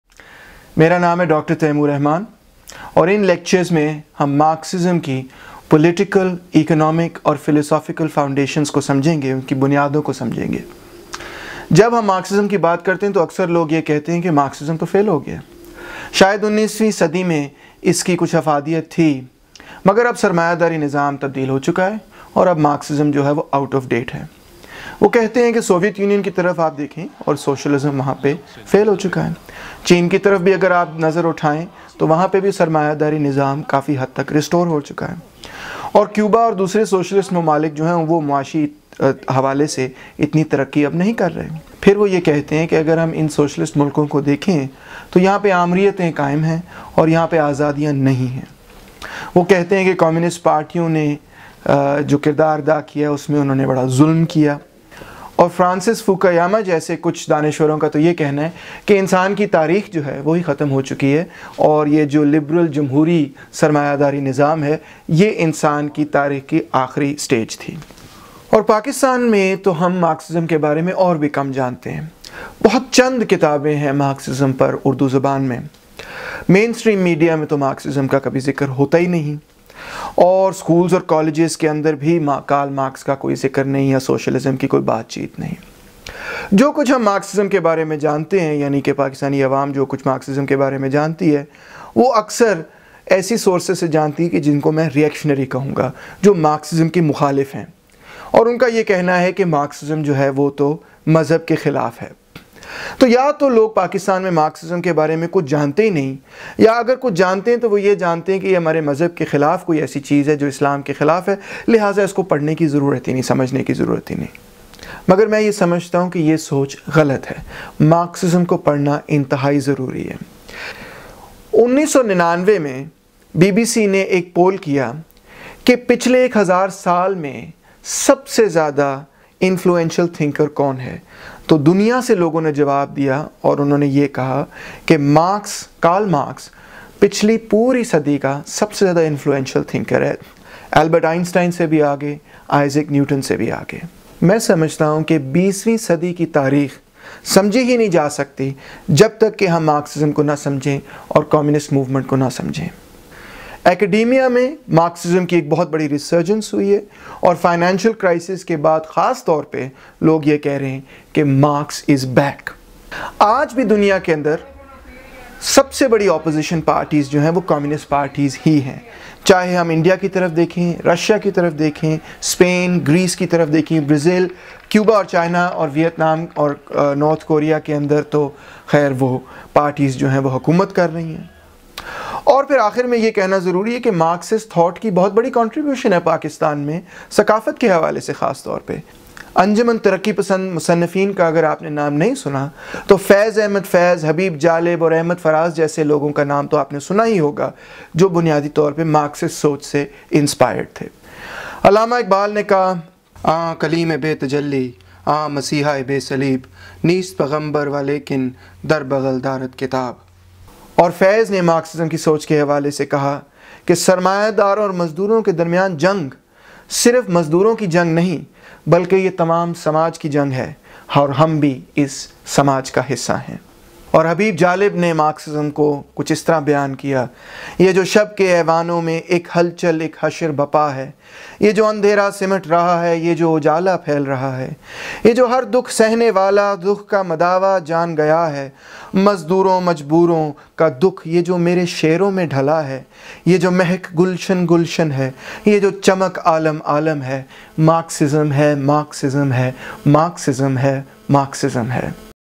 Why Study Marxism? (Urdu)
Dr. Taimur Rahman introduces the lecture series and explains why the ideas of Karl Marx must be studied.